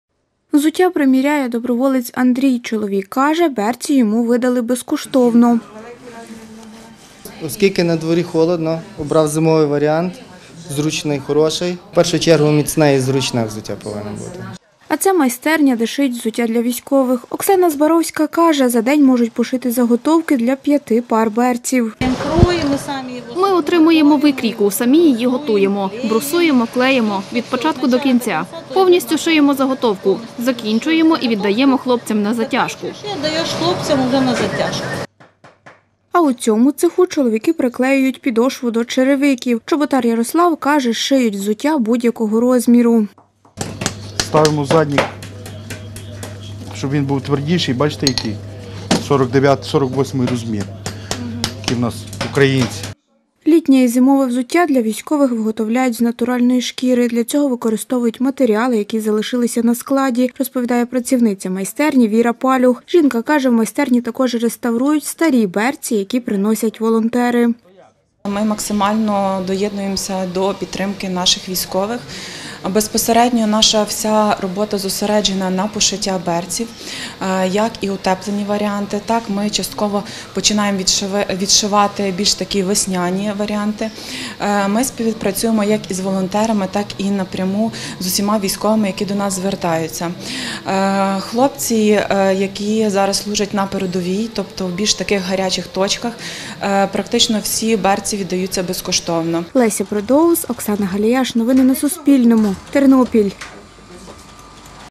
У Тернополі шиють берці для військових
Пів тисячі пар берців для військових пошили у взуттєвій майстерні в Тернополі. Про це розповіла Суспільному працівниця майстерні Віра Палюх.

За її словами, раніше в майстерні шили чоловіче і жіноче взуття. А з 2014 року, коли розпочалася російсько-українська війна, навчилися шити берці.